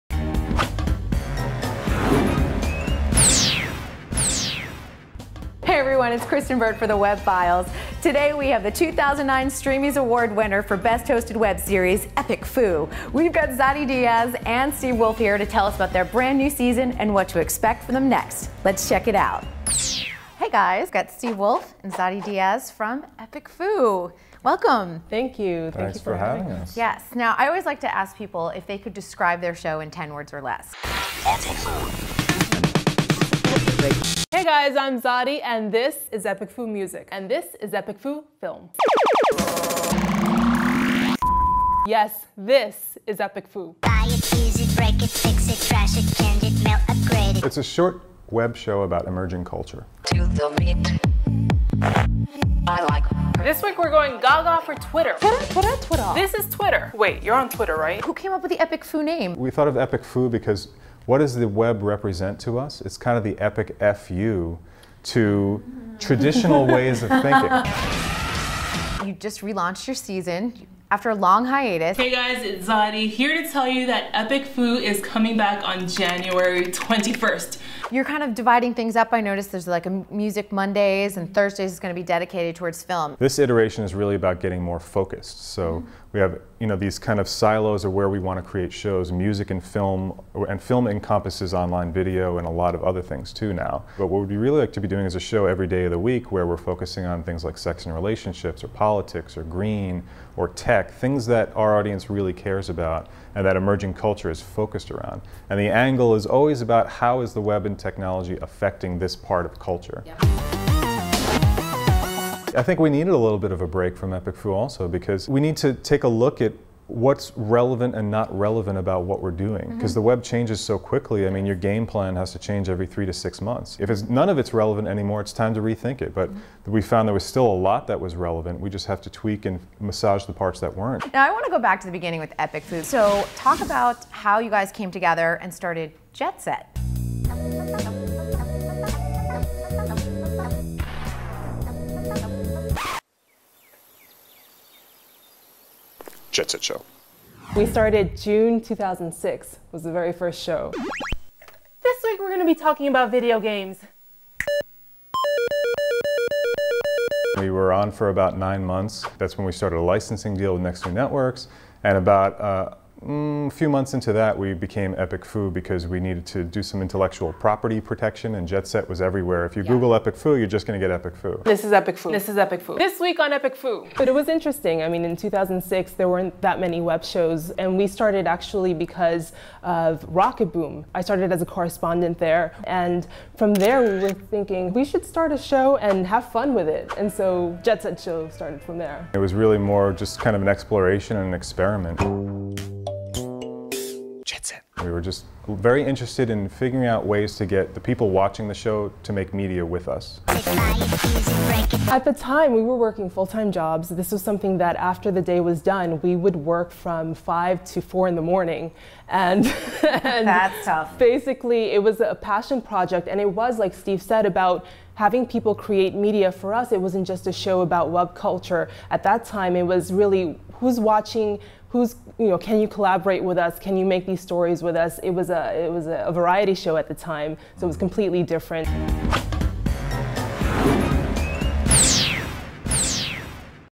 The Web.Files Snippet #6 - EPIC FU
Here are wonderful Zadi Diaz and Steve Woolf who created/hosted EPIC FU. They were the 2009 Streamy Award winners for Best Hosted Web Series, and Zadi took home a Streamy Award for Best Web Series Host in 2010.

Help us make TheWebFiles10!